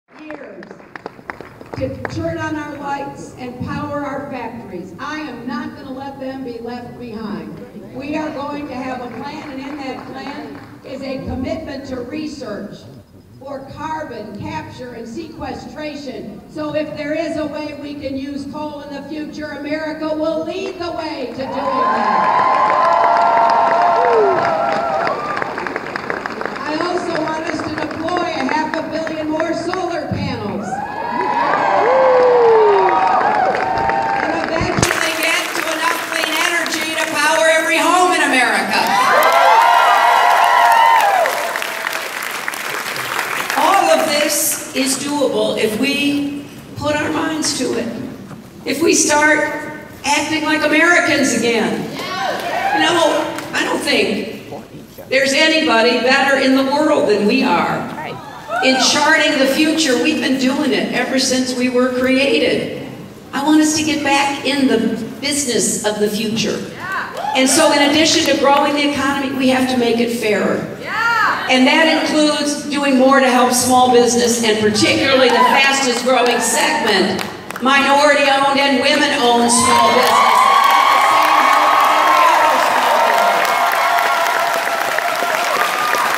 Hillary Clinton/ Lexington, KY Rally May 2016 " Coal/ Not Going to let them be left behind"
Hillary Clinton/ Lexington, KY Rally May 2016 " Coal/ Not Going let them be left behind"